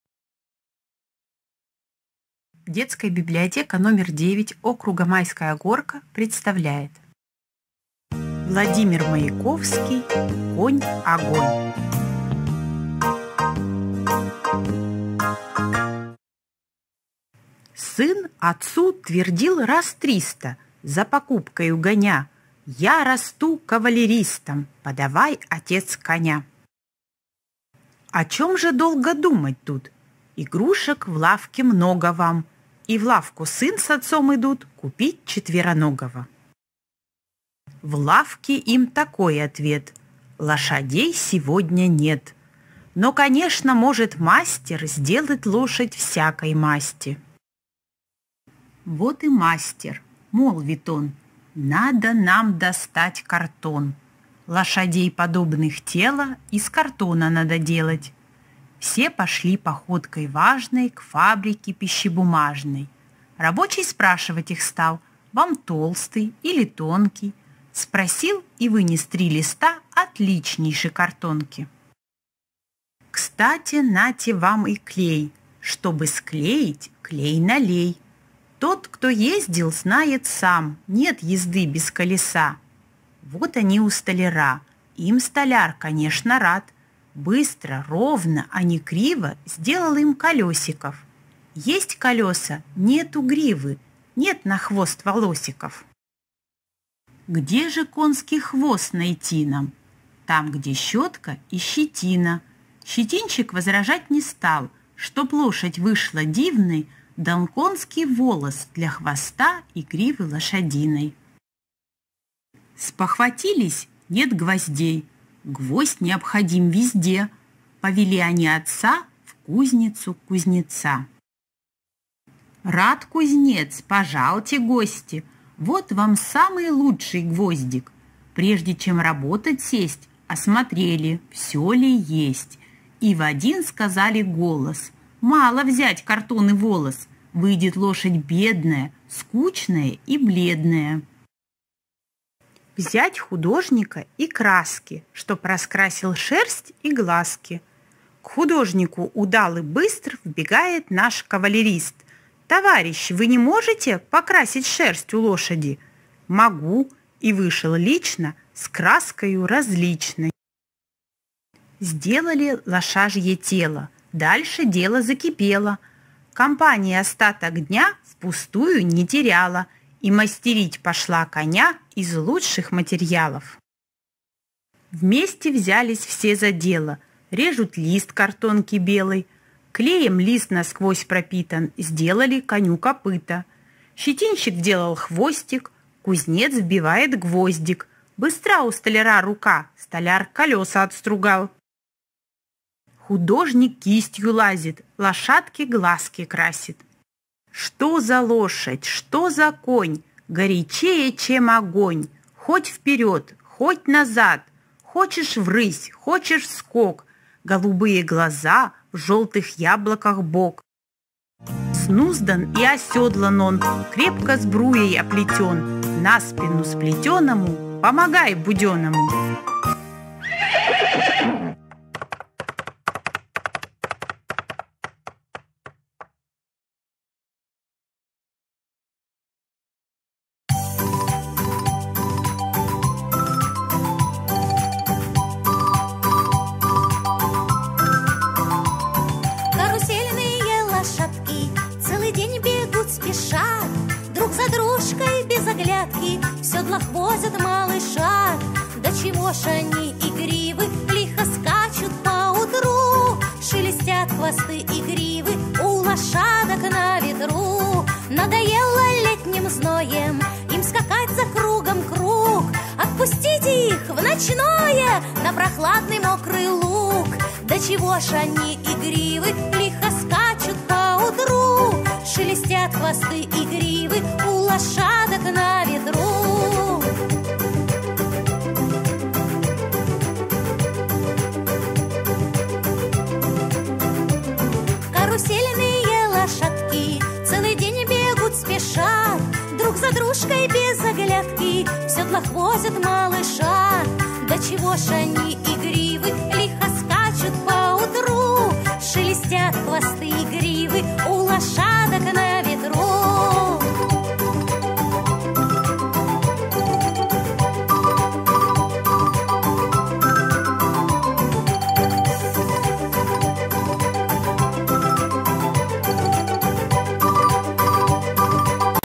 В. Маяковский «Конь-огонь»
В преддверии юбилея поэта Детская библиотека № 9 округа Майская горка предлагает послушать стихотворение и смастерить карусельных лошадок.